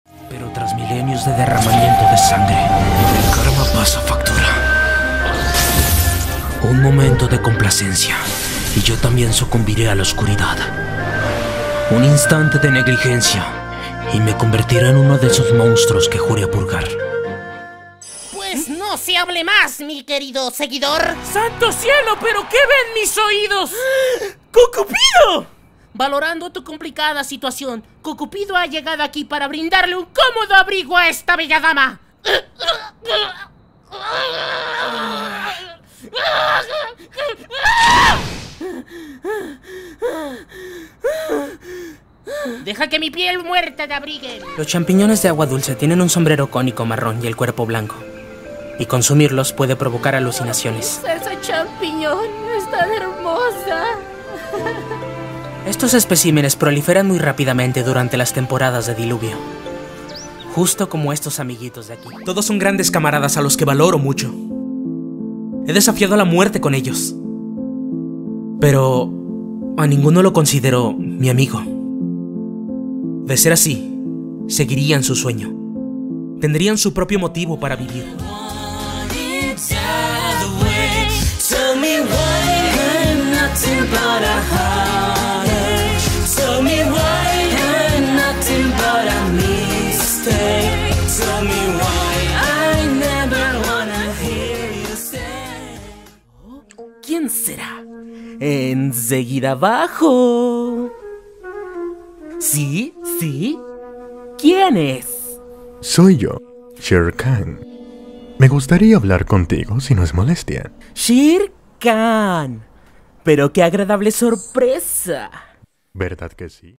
Alpa Kimeryo - Demito Express de voces
Hola no tengo mucha material que me convenza como las piezas selecionadas aqui, ademas que tecnicamente son los mas recientes todo lo que esta atras no deberia existir por favor borren eso.